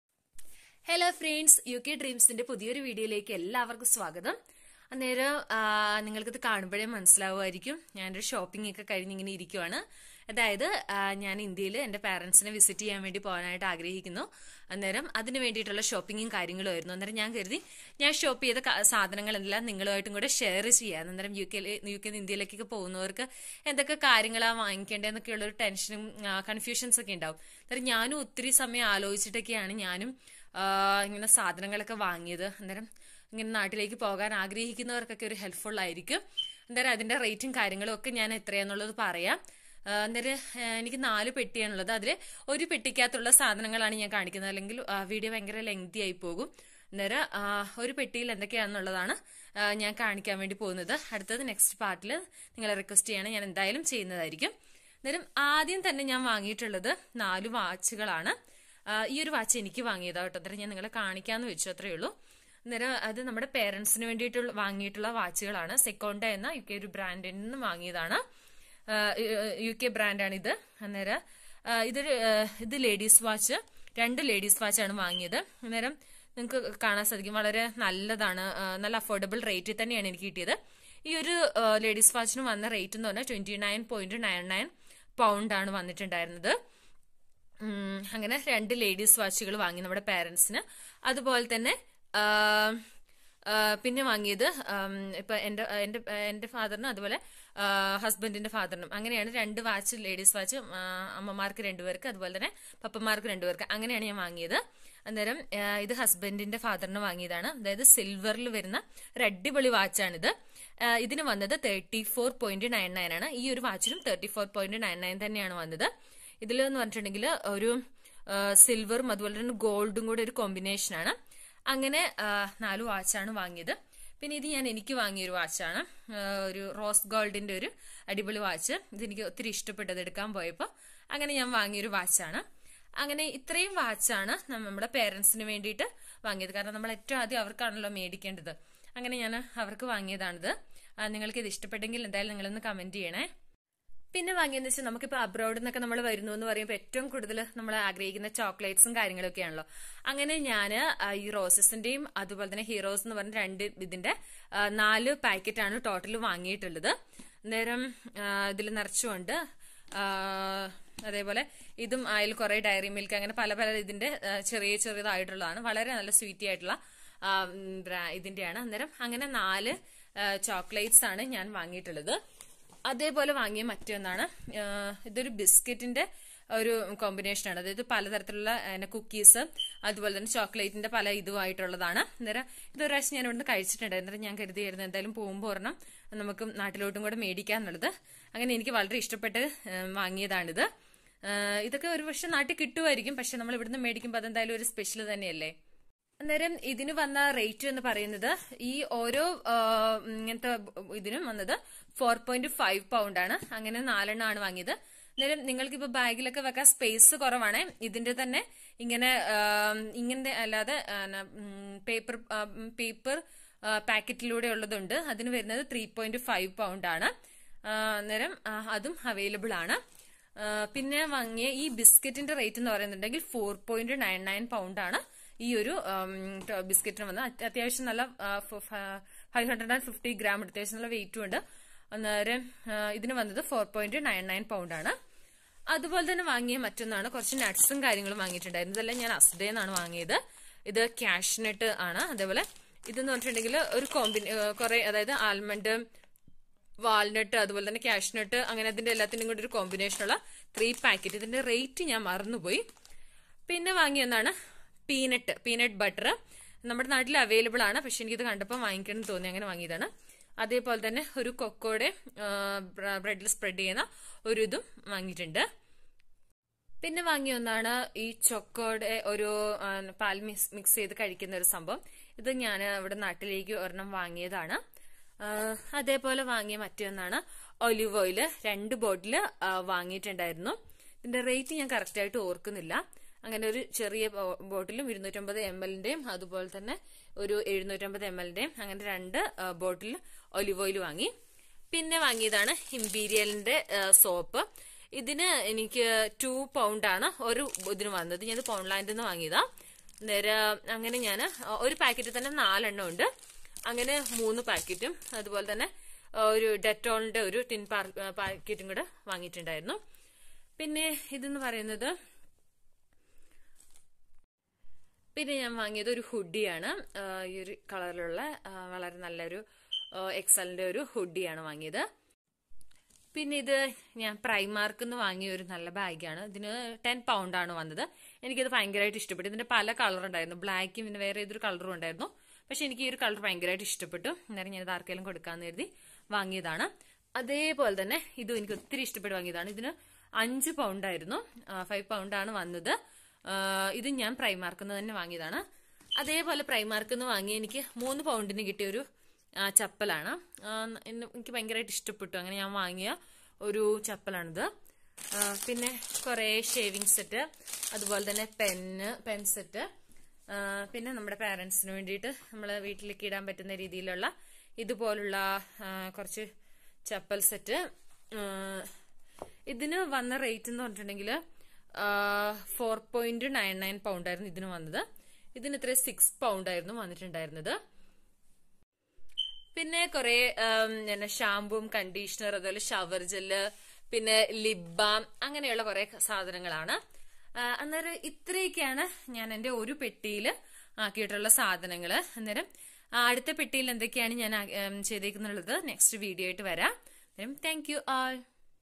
UK to India shopping vlog/Gifts from UK to India/Gifts for friends and family from London/Part 1
in this video I would like to explain my small shopping gifts from UK to India.